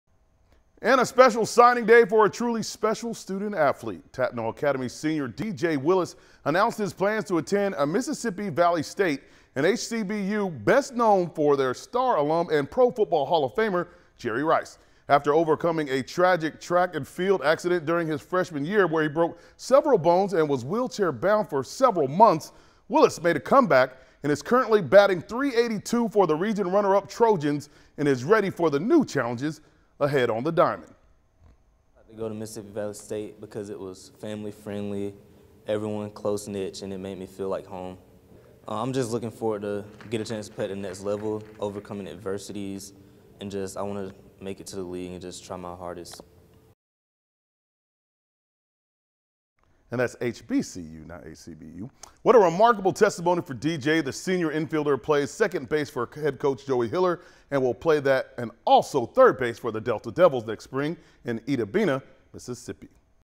Tattnall's DJ Willis signs with Mississippi Valley State
DJ Willis announced his plans to attend Mississippi Valley State, an HBCU best known for their star alum and Pro football Hall of Famer Jerry Rice

13WMAZ News is the #1 station for news and weather in the Central Georgia area, serving people in Dublin, Macon, Milledgeville and Warner Robins.